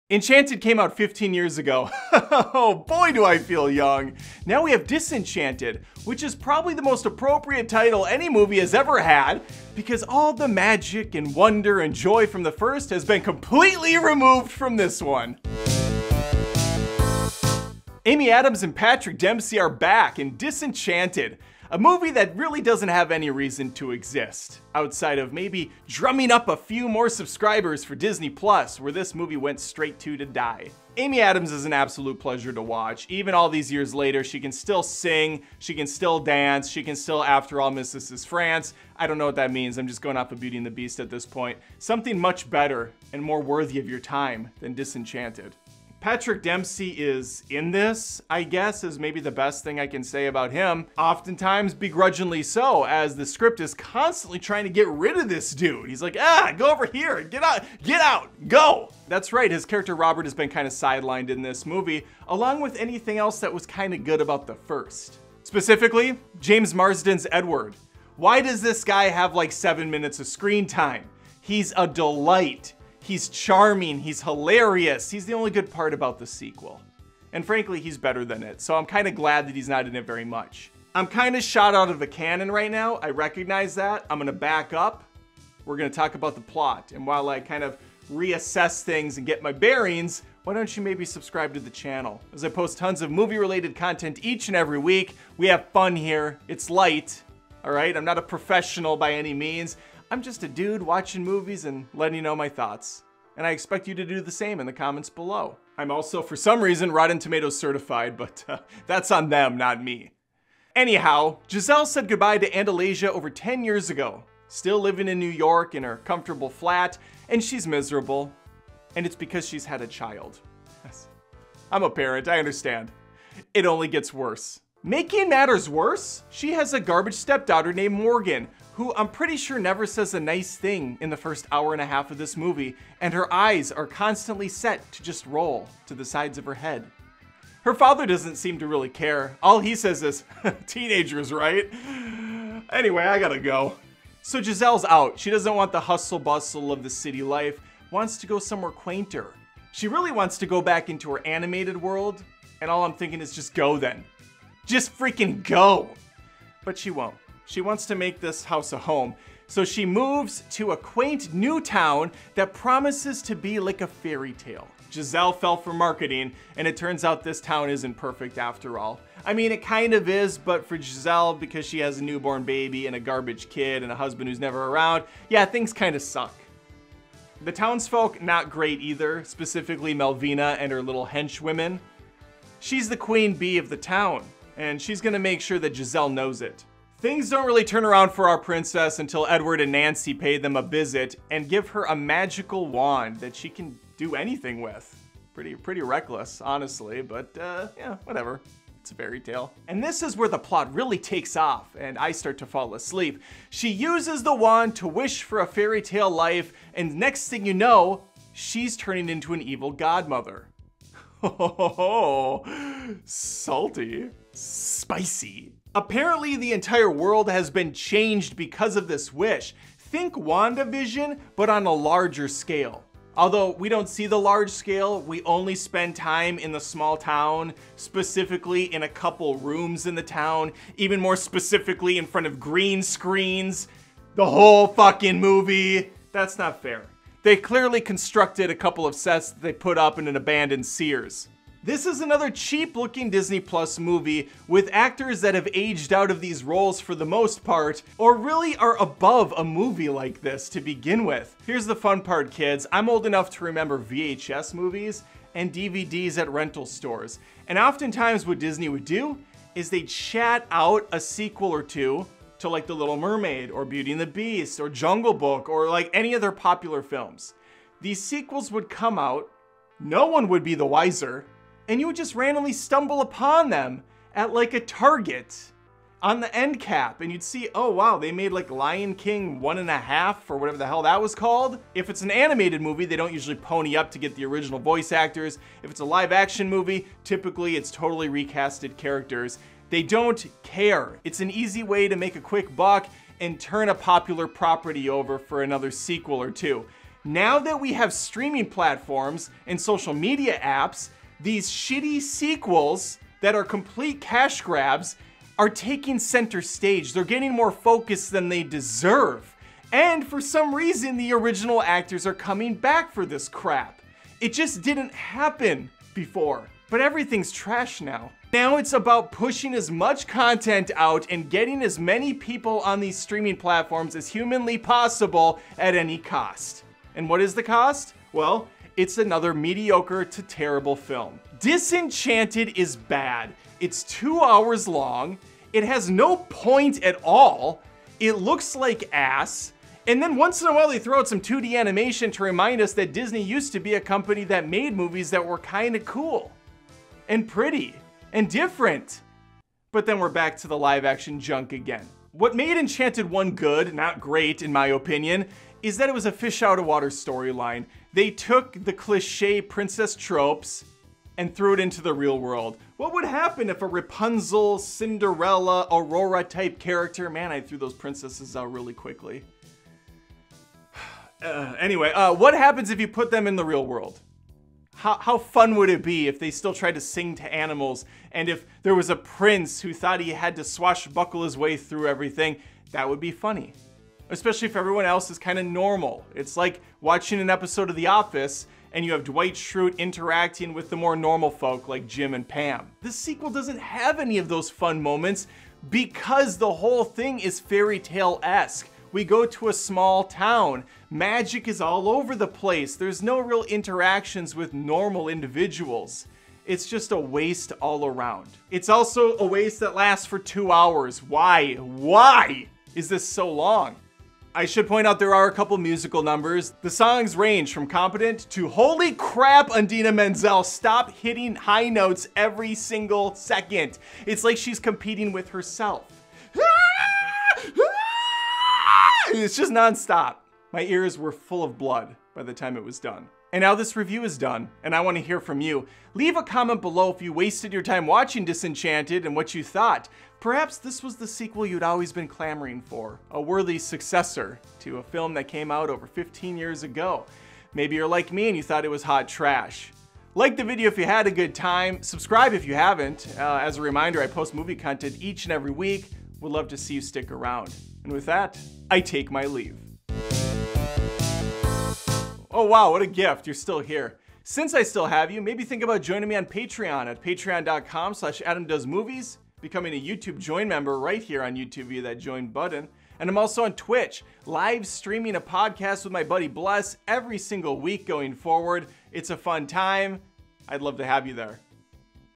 Disenchanted Review - Another Crappy Disney+ Movie
Disenchanted movie review. Enchanted came out 15 years ago so naturally Disney decided now is the perfect time to ruin another classic. Enchanted 2 (Disenchanted) brings back all the favorites from the first and either doesn't use their talents or changes their personalities entirely. It's not an overall failure but Disenchanted is certainly a waste of time and talent.